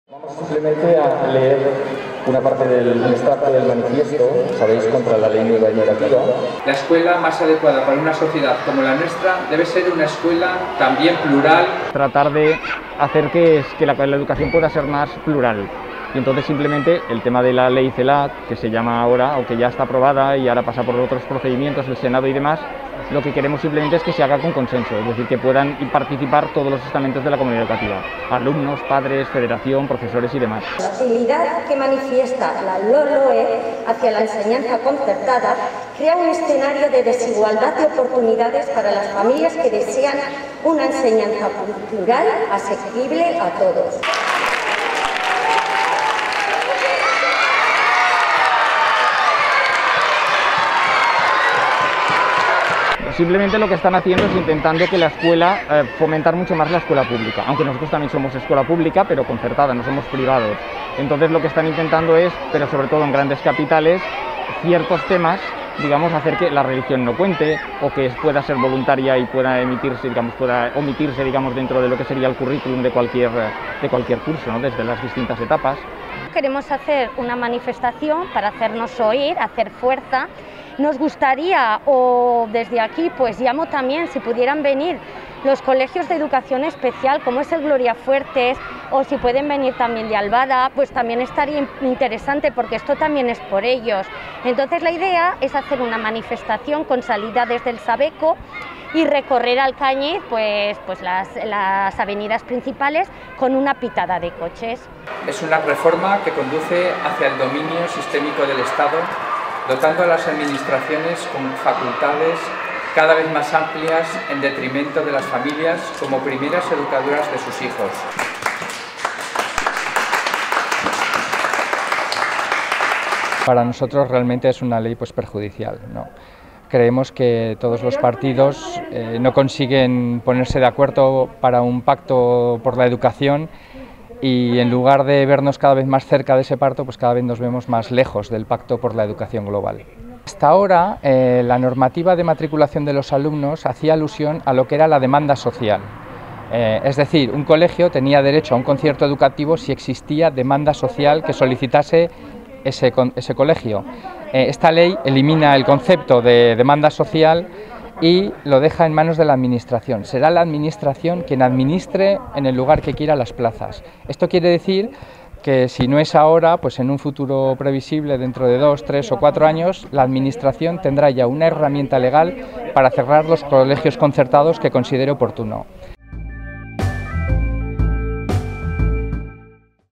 Movilización de los colegios concertados de Alcañiz: Stop Ley Celaá
Los colegios concertados de Alcañiz, La Inmaculada y San Valero, se han movilizado en la mañana del viernes 20 de noviembre en contra de la nueva ley Celaá. Se ha leído el mismo manifiesto en los dos centros y se han lanzado globos ecológicos de color naranja . Se están preparando nuevas movilizaciones para el próximo domingo en Alcañiz.